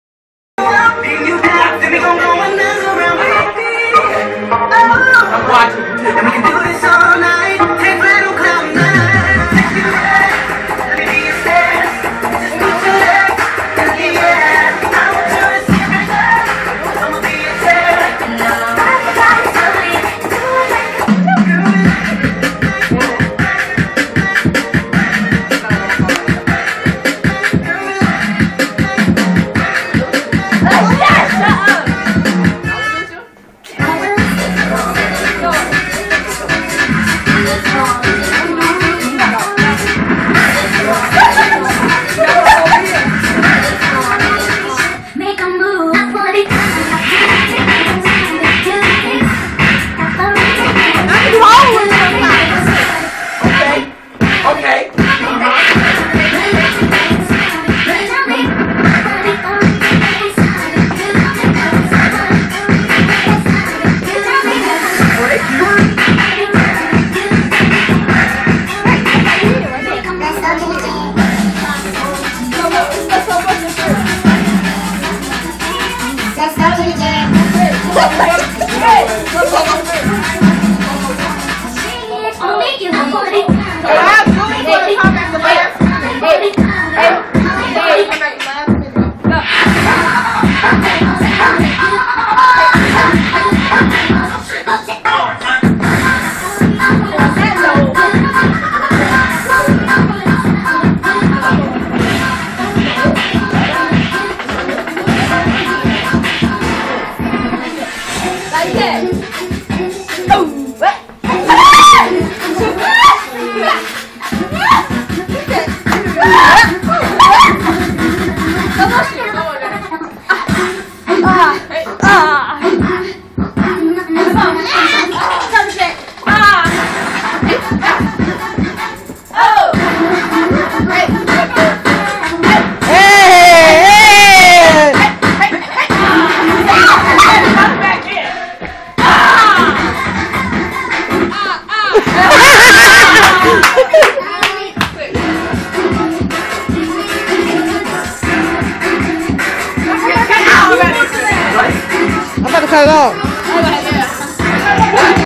JERSEY CLUB freestyling
Show Stoppaz/ D & G just having FUN after practice!!!